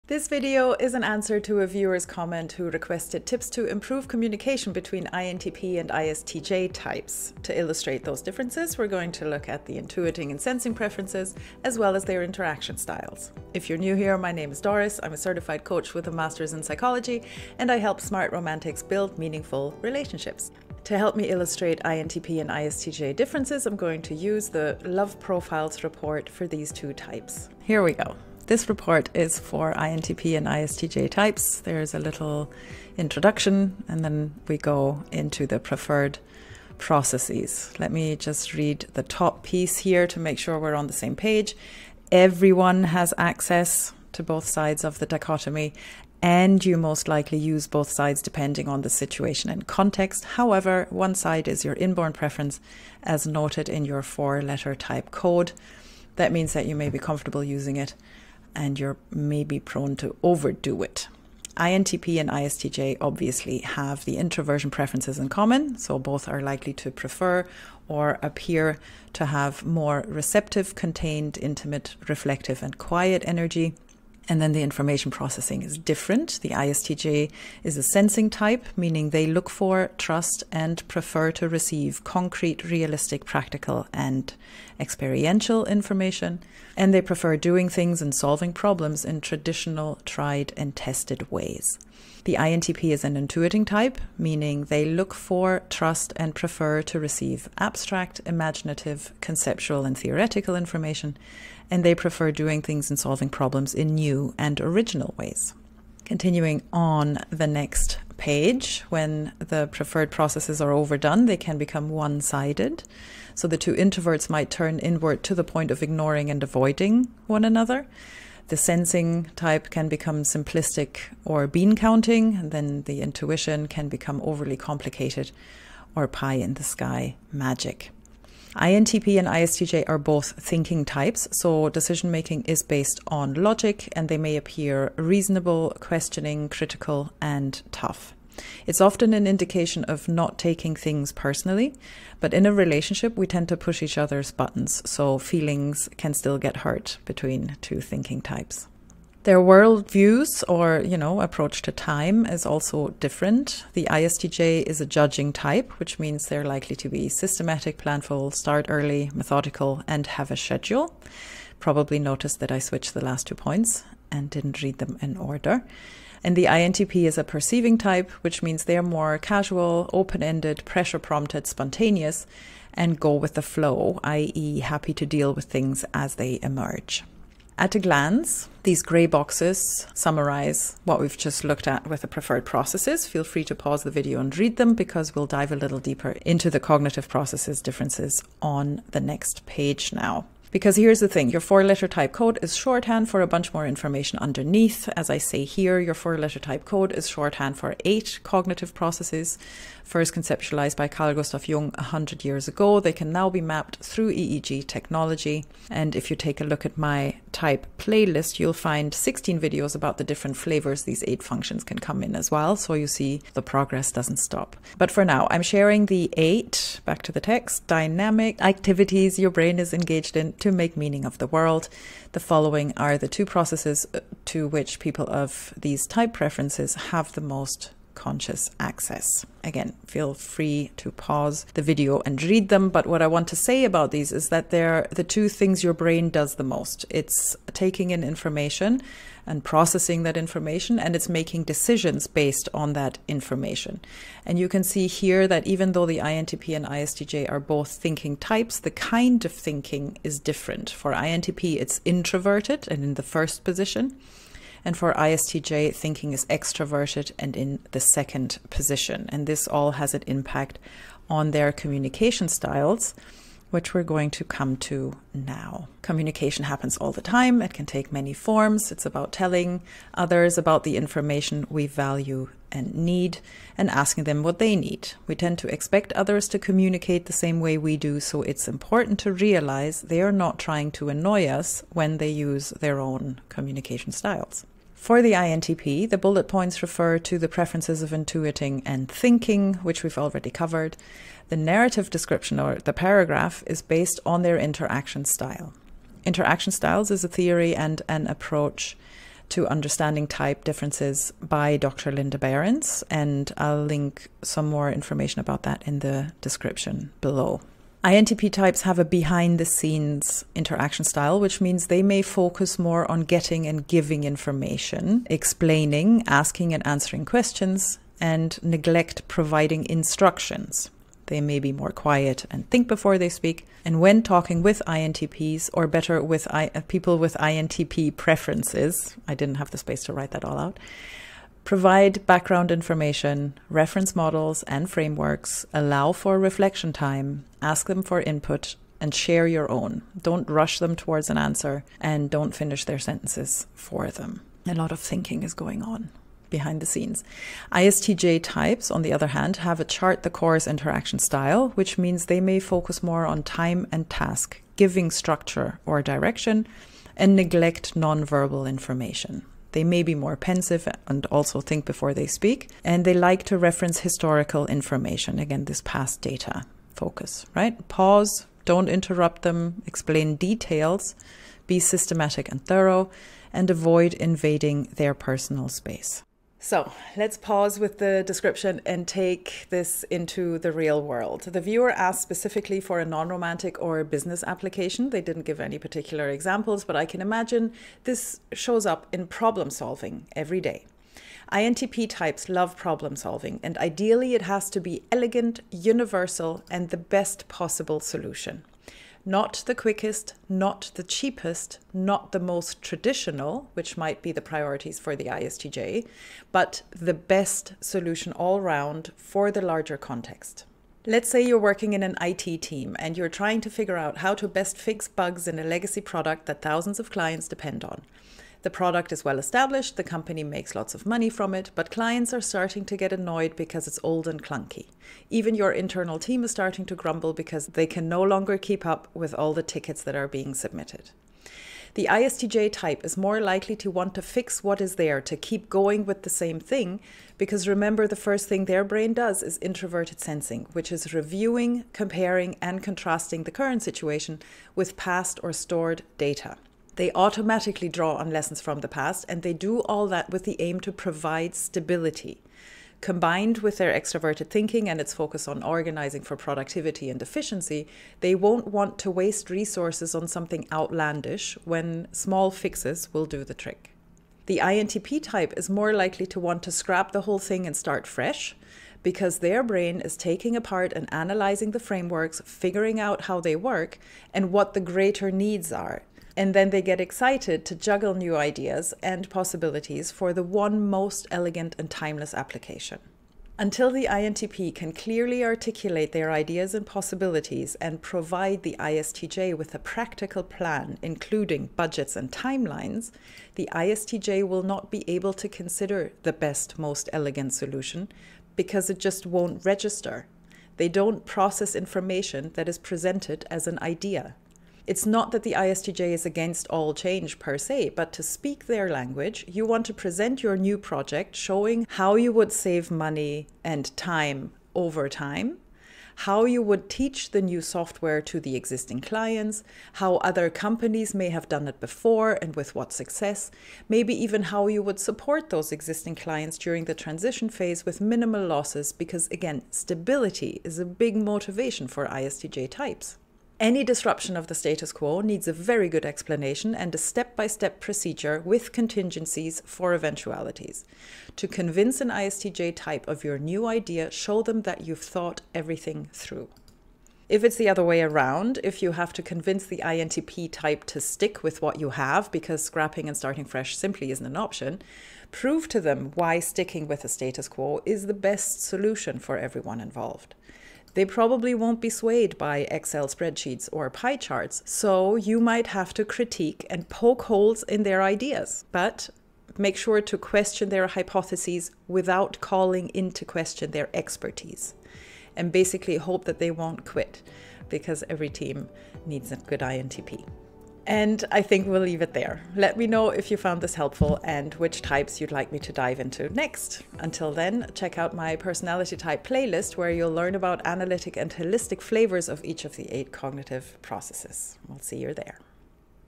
How to get your point across to INTP and ISTJ, and make yourself heard
How do you convince your ISTJ boss of your great new idea?
Why are INTP Types so indirect?

In this video, I'll take you through a side-by-side comparison of communication styles between INTP and ISTJ, and give a work example in the end.

In this video:
00:27 - INTP-ISTJ Love Profiles
01:04 - Introversion
01:16 - Sensing
01:33 - Intuiting
01:51 - One-sided traps
02:13 - Thinking
02:34 - Judging
02:47 - Perceiving
03:01 - At a glance
03:29 - Cognitive Dynamics
03:53 - Different kinds of Thinking
04:24 - Communication Styles
05:09 - INTP Behind-The-Scenes™ Communication Style
05:48 - ISTJ Chart-the-Course™ Communication Style
06:22 - Example: Problem-Solving in an IT team